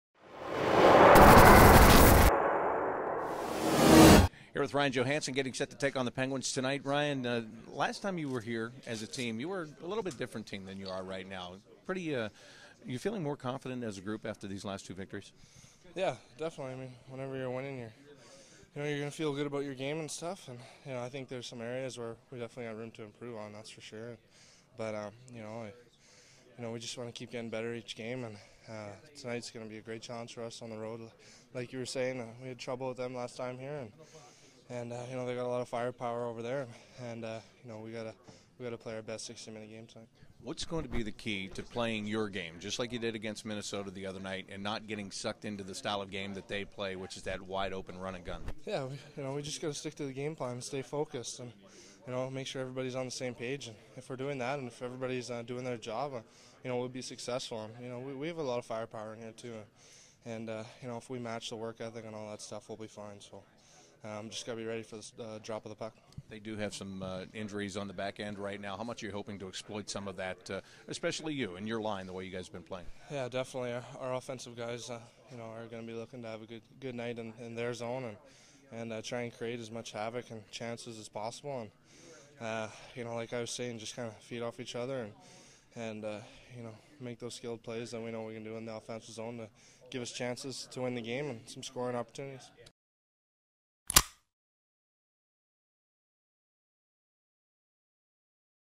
Pregame Interview: Ryan Johansen
Ryan Johansen pregame interview before the Blue Jackets face-off against the Penguins on the road in Pittsburgh.